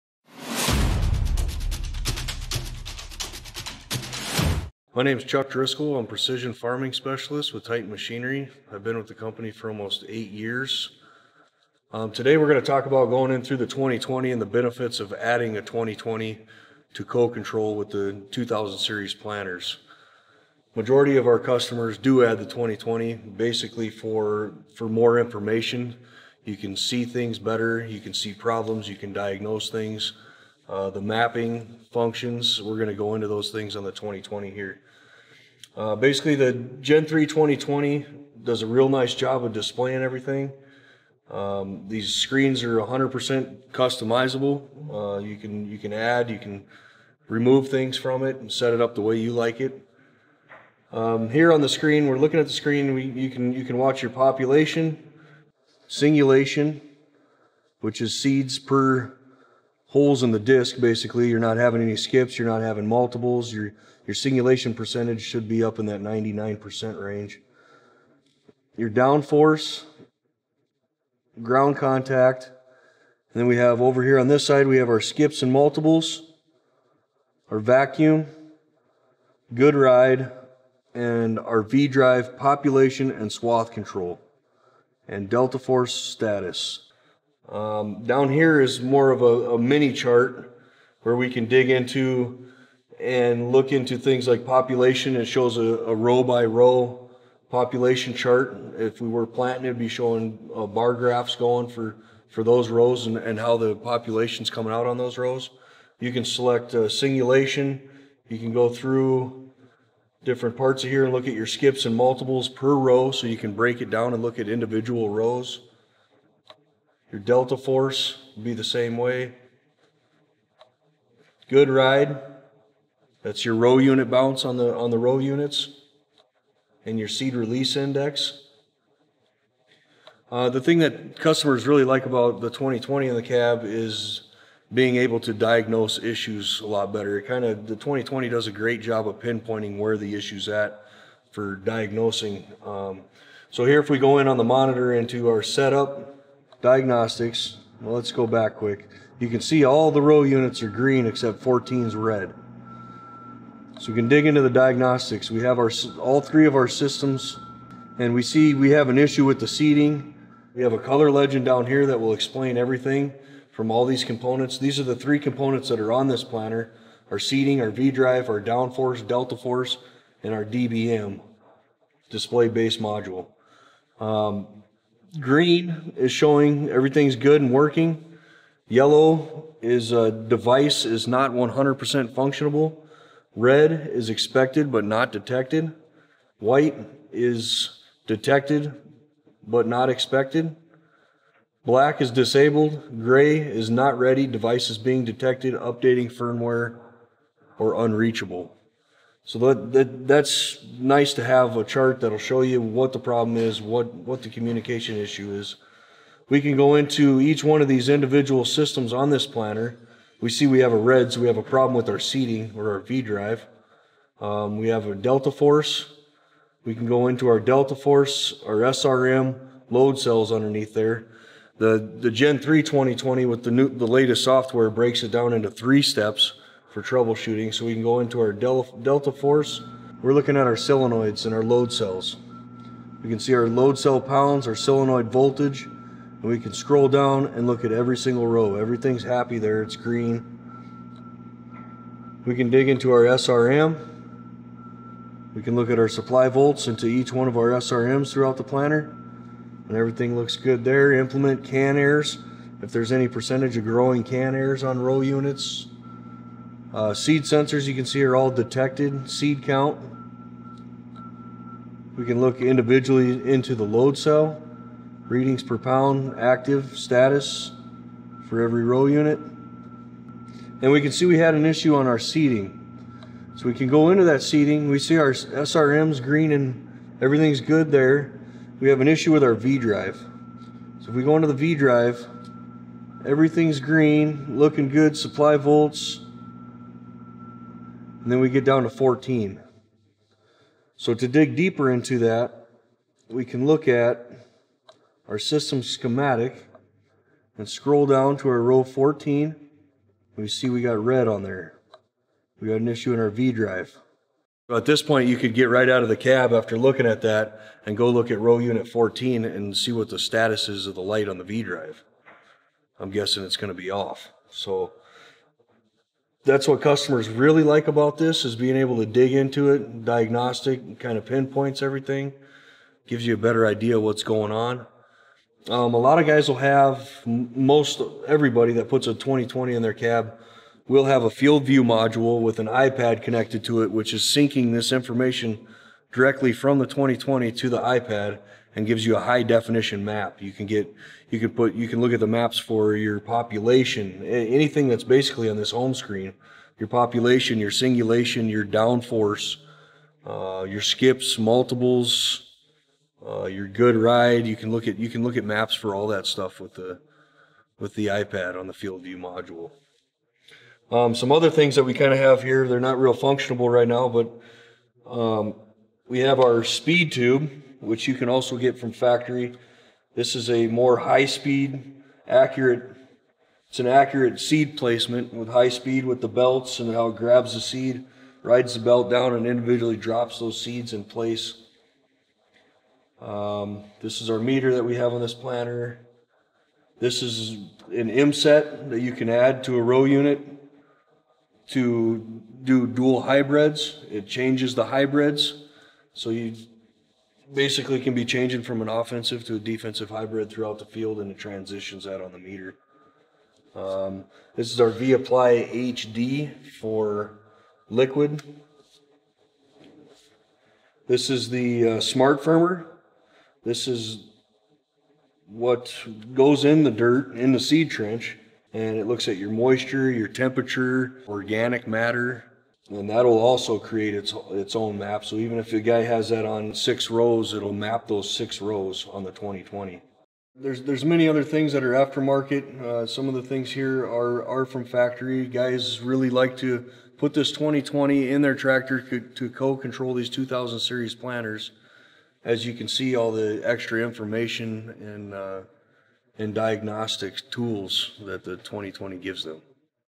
20|20 Precision Planting Monitor Overview | Titan Machinery
An experienced Titan Machinery Precision Farming Specialist dives into the features and benefits of adding a 20|20 Monitor to co-control with your 2000 Series Case IH planter. Learn about customization options and how to trouble shoot and diagnose issues on your planter from the cab with the 20|20 Precision Planting monitor.